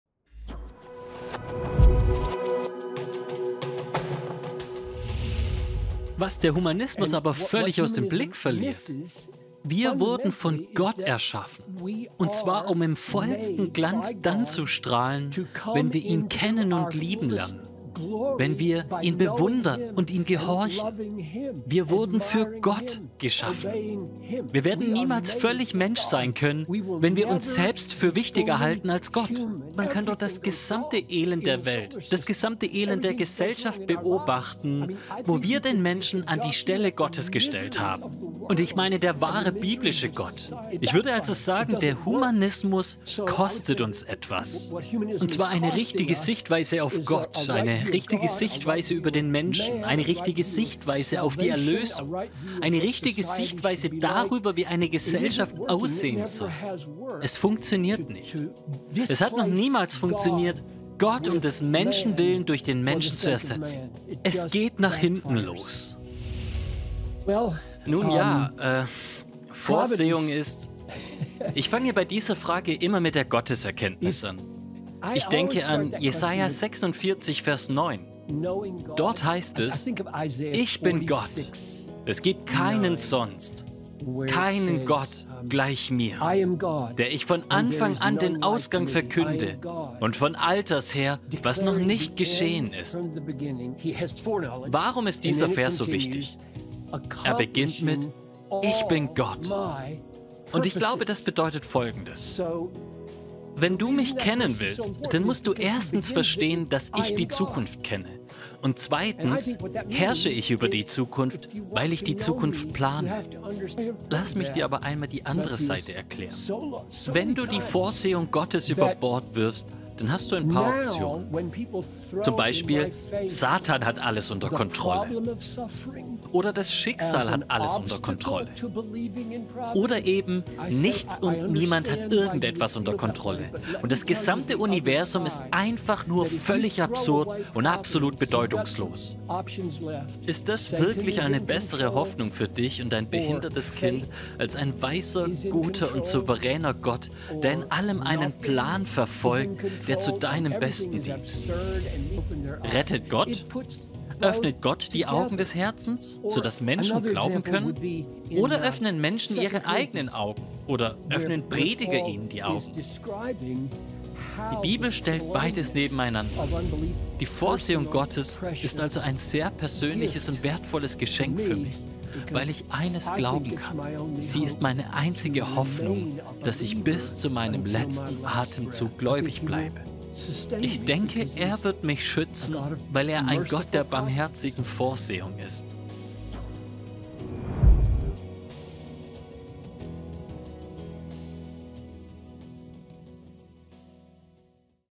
Die Vorsehung Gottes | John Piper | Auf den Punkt gebracht | offen.bar
John Piper im Interview! Hier ein kleiner Ausschnitt. Du merkst, es handelt sich um spannende Themen, wie um die Vorsehung Gottes, Leid, den Humanismus und weitere Inhalte! Bleib gespannt. Ganzes Interview is coming soon…